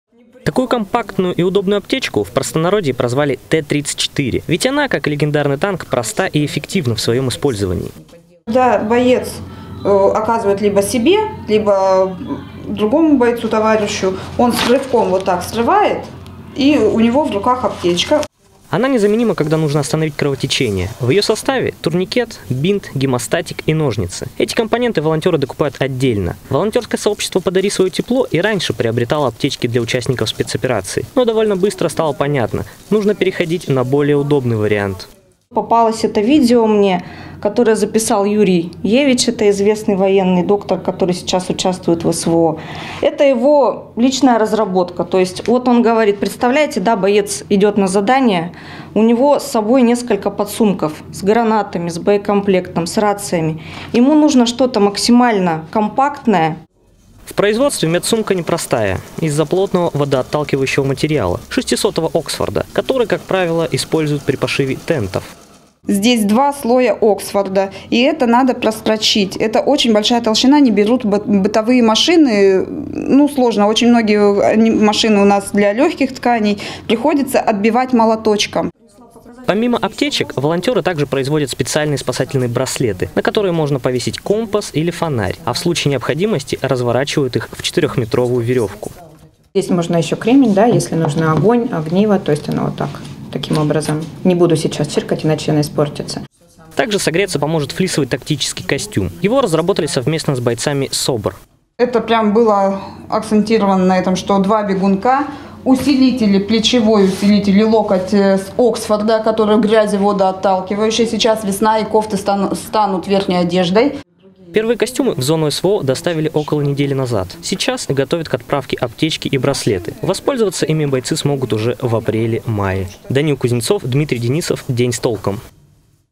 Волонтеры Алтайского края налаживают производство индивидуальных аптечек для участников СВО
Медсумка Т-34, как и легендарный танк, проста и эффективна в своем использовании. Производство индивидуальных аптечек, тактических костюмов и браслетов для участников СВО налаживают волонтеры Алтайского края. Часть экспериментального снаряжения уже испытывают на фронте.